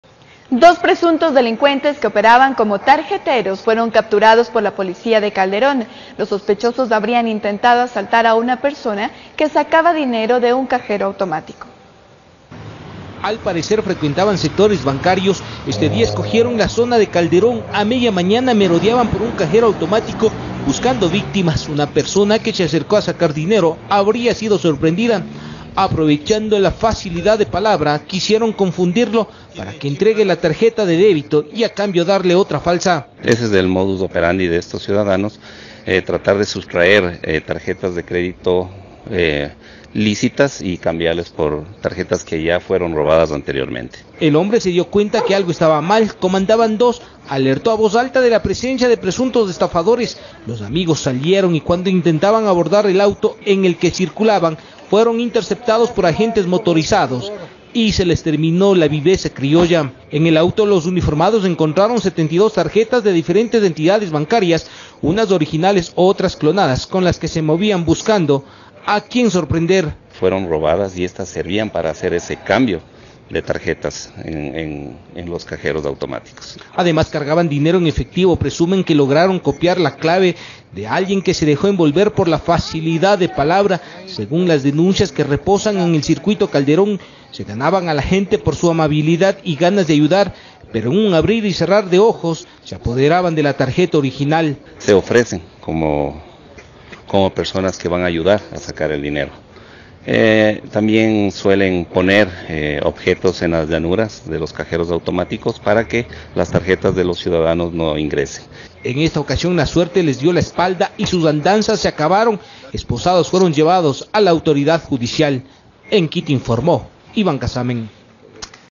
Policía de Calderón detiene a dos presuntos delincuentes
Operaban como tarjeteros